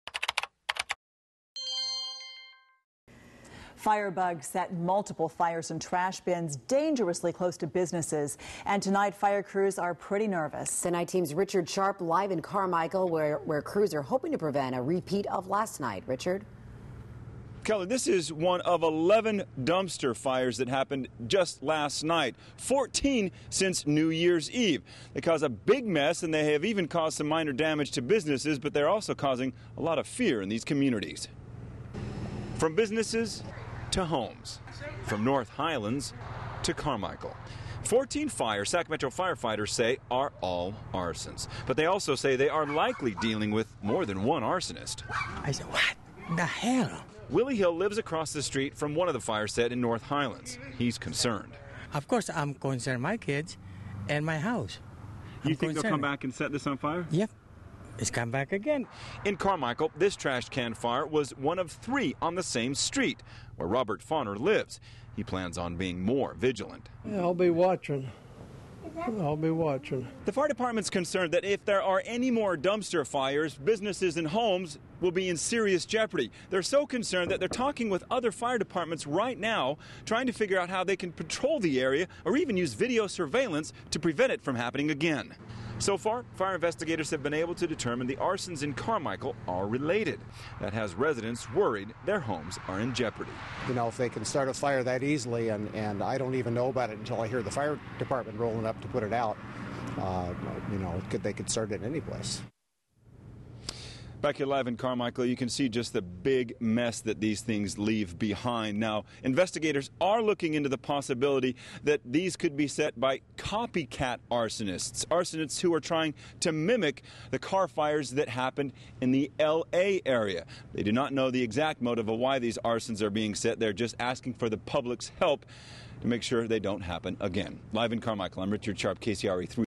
Fire Investigators Searching For Arsonist
Investigators say the fires in Carmichael are related.